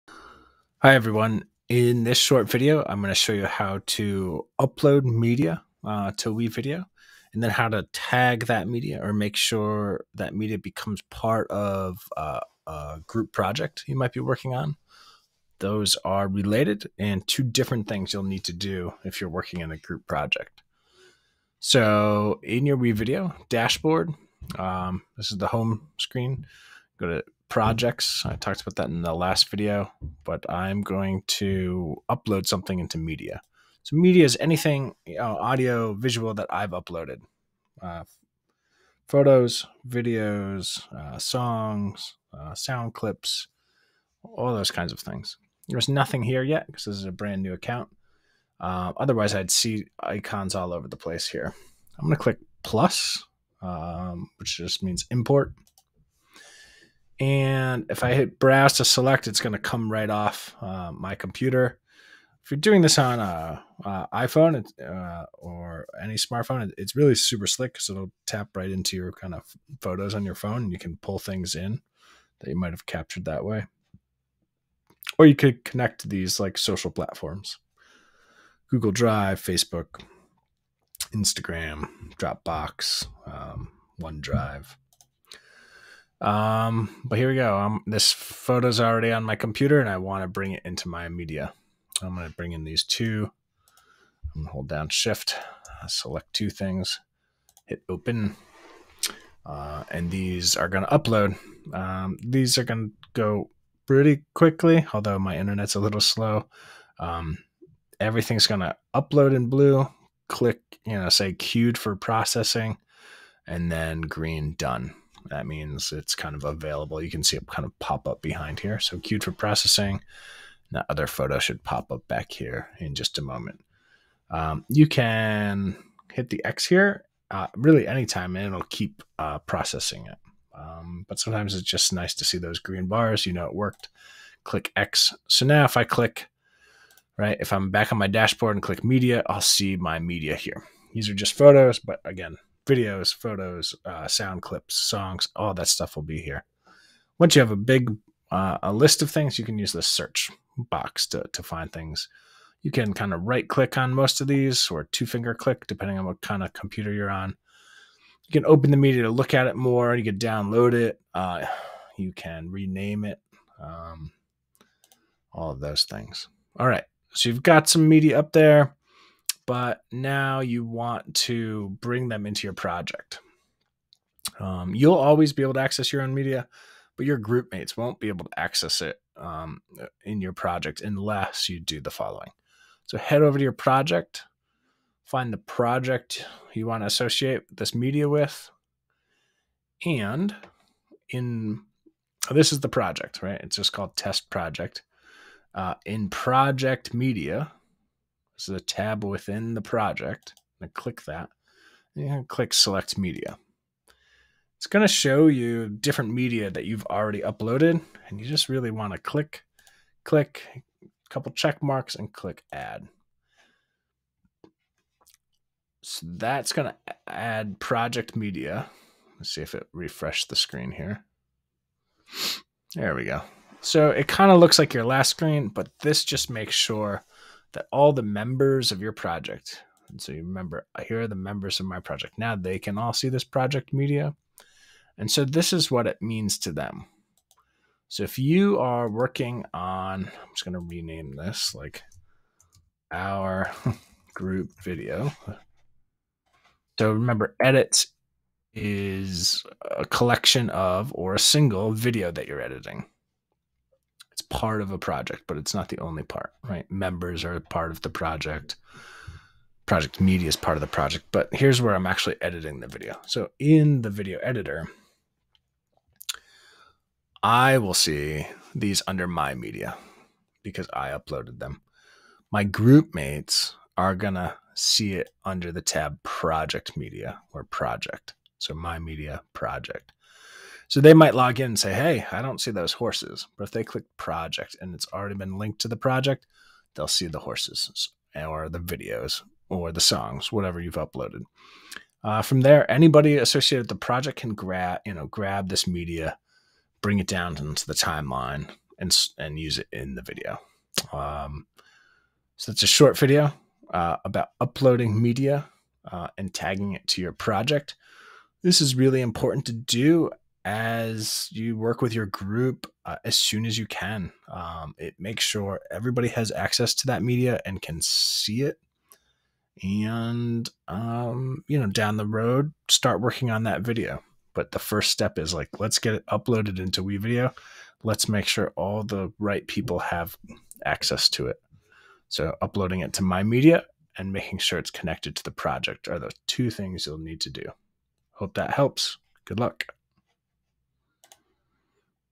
WeVideo: How to upload media and make sure others can see and use that media in our group project.
In this short tutorial, we explore how to upload media. Media is any audio/visual content that you have created or found and want to use in your video. This is often video recordings, photographs, and sound/song files.

Step one: Upload media.
Step two: Connect uploaded media to your group project.

Make sure to do both steps or your groupmates won't see the files they are looking for!